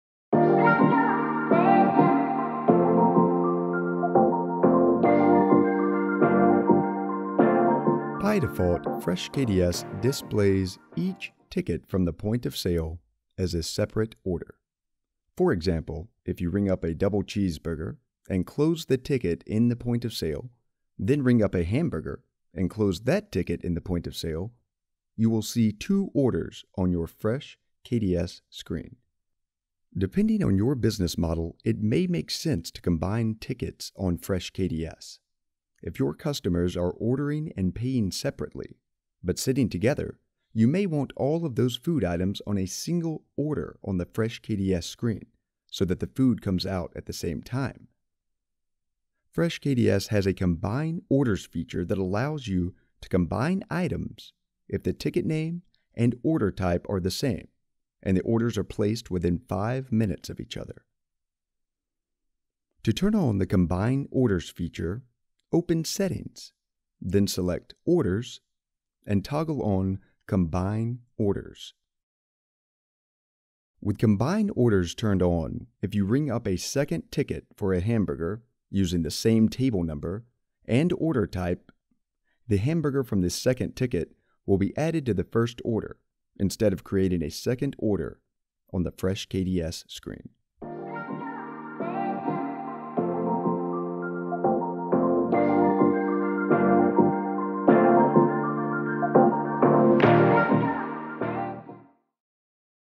Fresh KDS Training - Combine Orders
Fresh KDS gives you the ability to combine orders that have the same ticket name and order type.  Learn how to combine a new order with an existing order on the Fresh KDS screen.